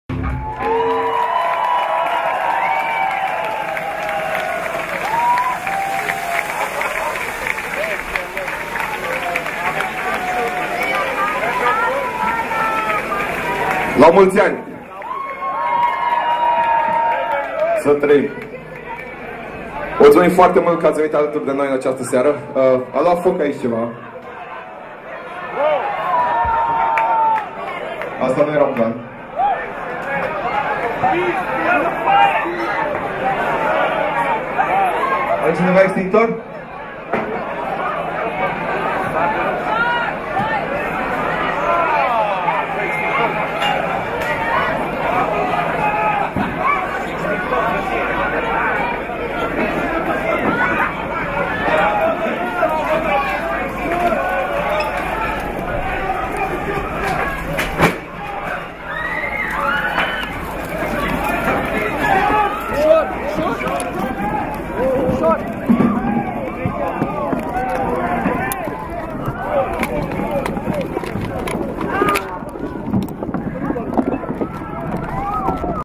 Filmare socanta din clubul Colectiv - Flacarile albastre
Avem marturiile incendiare ale unor supravietuitori din infernul de la Clubul Colectiv care confirma ceea ce deja multe persoane suspecteaza in legatura cu incendiul din seara de 30 octombrie 2015, in care au murit peste 60 de persoane: deflagratia din clubul Colectiv nu putea fi rezultatul unui "foc de burete", ci a prezentei unor substante neidenficate inca.